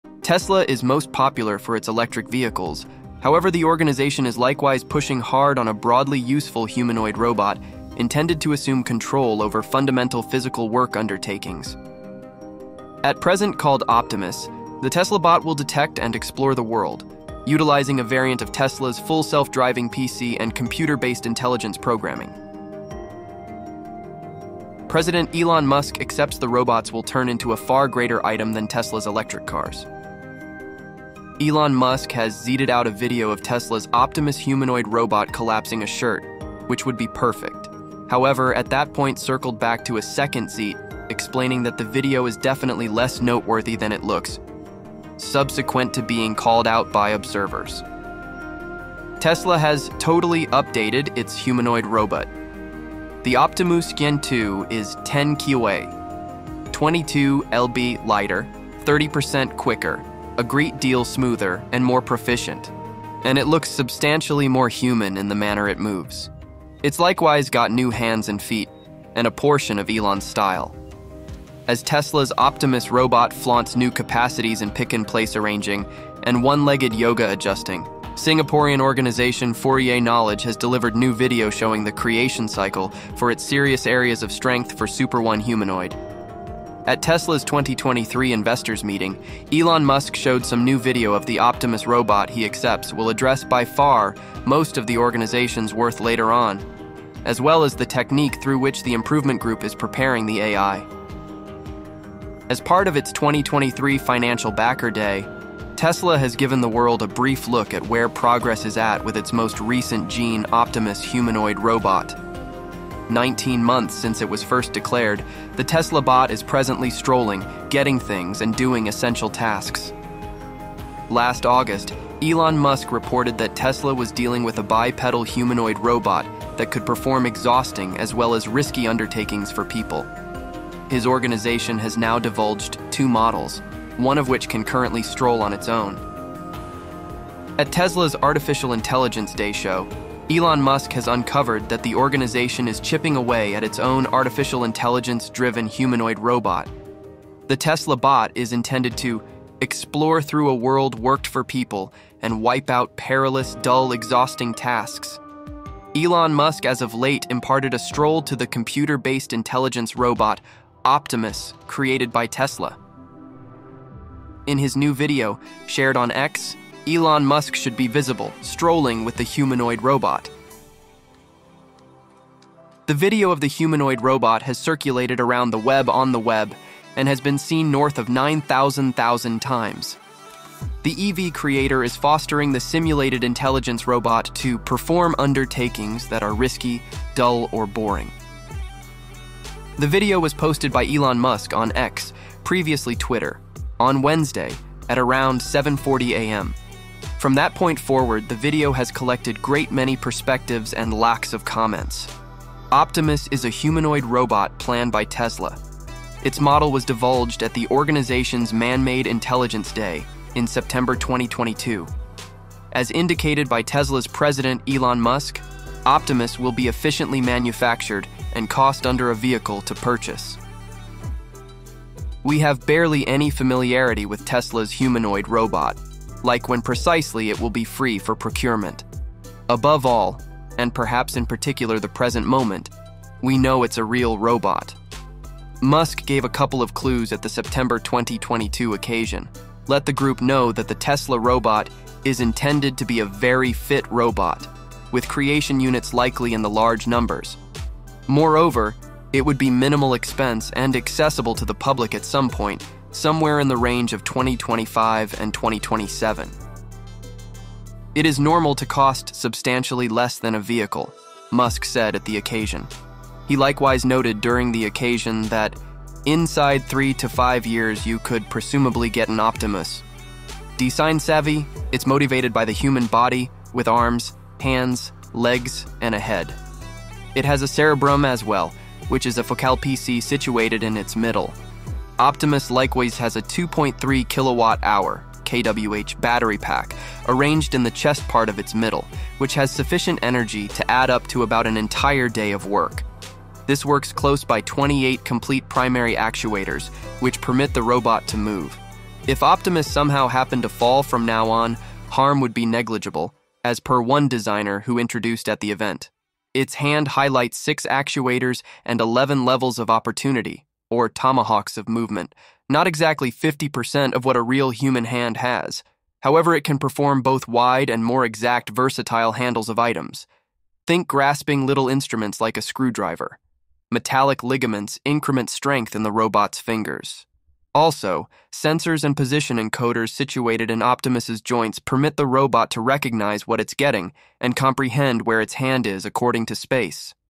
Elon Musk Reveals New OPTİMUS GEN-2 Robort video!
Get ready for the future as Elon Musk unveils the groundbreaking Tesla Bot! In this exclusive reveal, Musk introduces Optimus, the humanoid robot set to revolutionize industries. Join us as we delve into the latest Tesla news and Elon Musk's vision for the future of robotics. From Tesla's innovative technology to the implications of AI robots, discover how the Tesla Bot is poised to transform our world. Don't miss out on this game-changing announcement from the mastermind behind Tesla and SpaceX! ElonMusk Tesla TeslaBot Optimus HumanoidRobot AI Robotics"